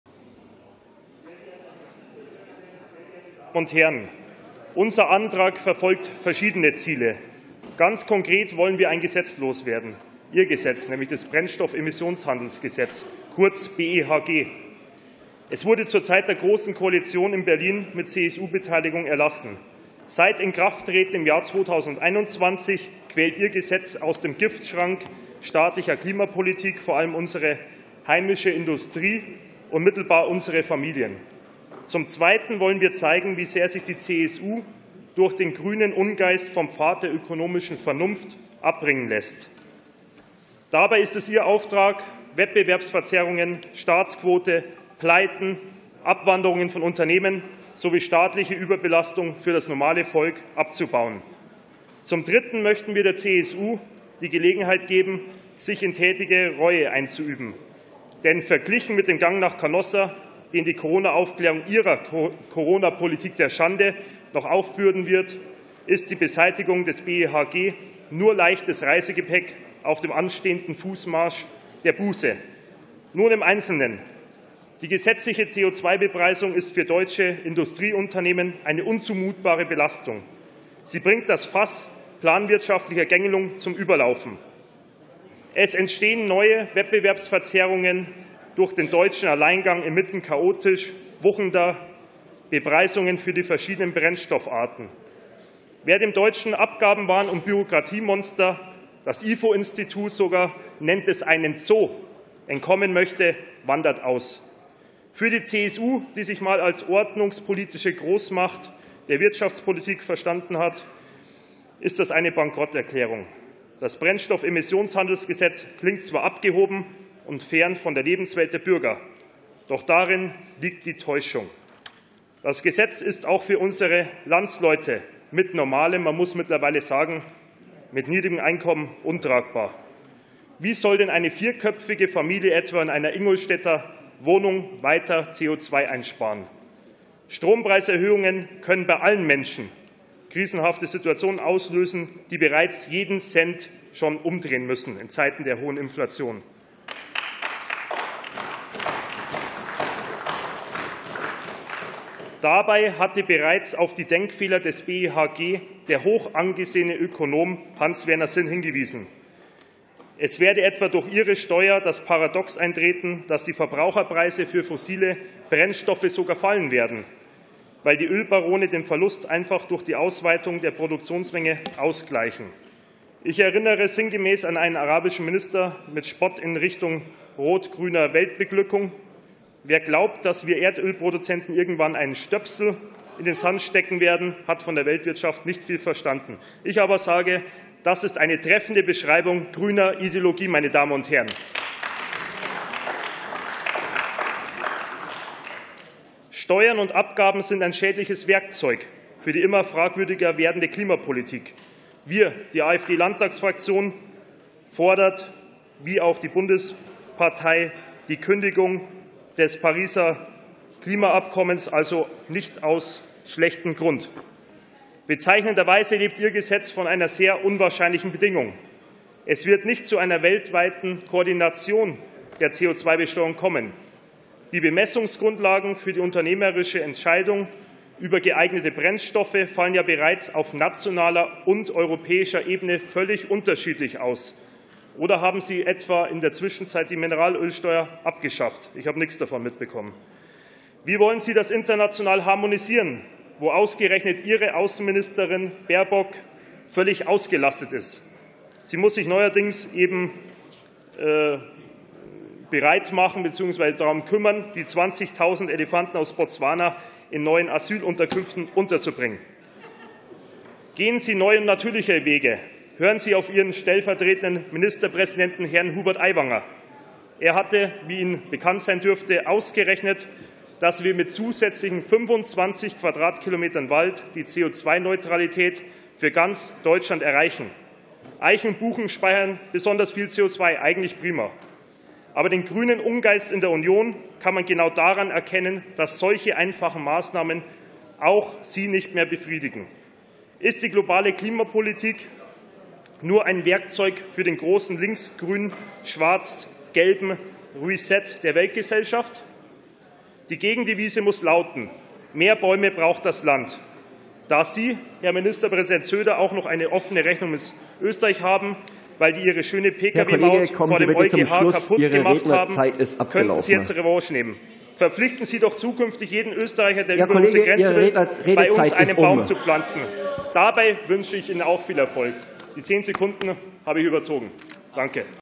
Wirtschaft retten - Standort sichern - CO2-Steuern abschaffen
▶ Oskar Lipp mit klarer Botschaft: Wirtschaft retten - Standort sichern - CO2-Steuern abschaffen